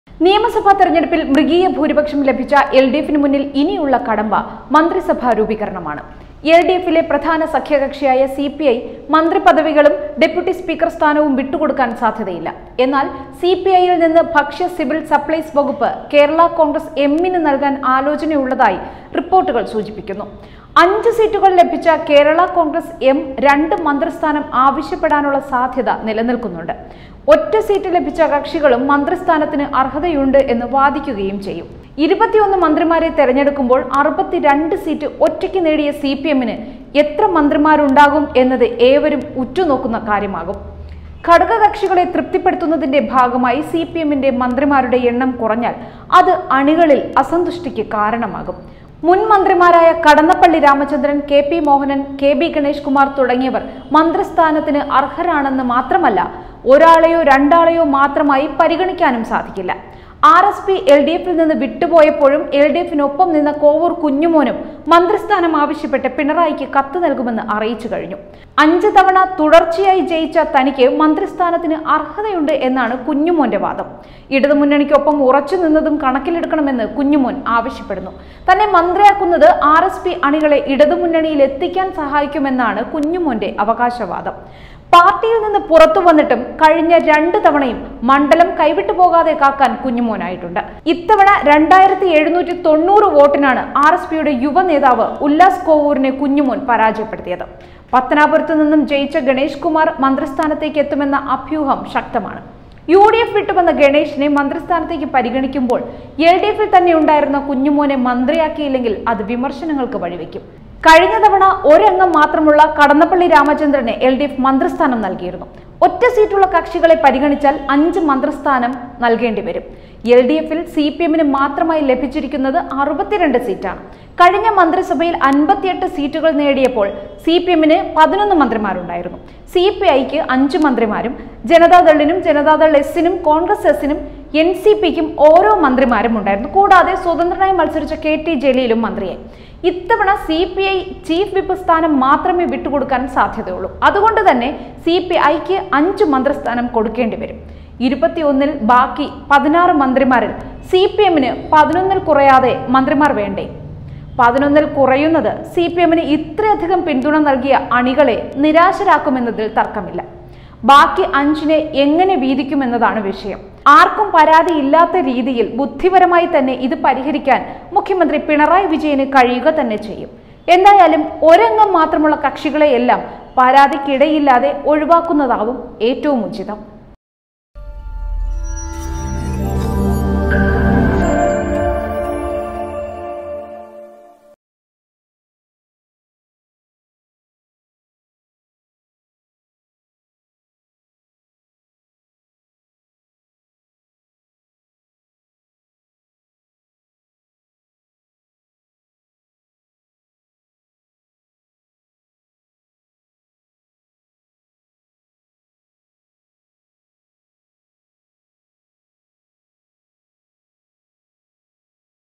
സിപിഐ മന്ത്രിസ്ഥാനം വിട്ടുകൊടുക്കില്ല;സിപിഎം മന്ത്രിമാരുടെ എണ്ണം കുറയ്ക്കാനാവില്ല
സഖ്യകക്ഷികളെ എങ്ങനെ തൃപ്തിപ്പെടുത്താം?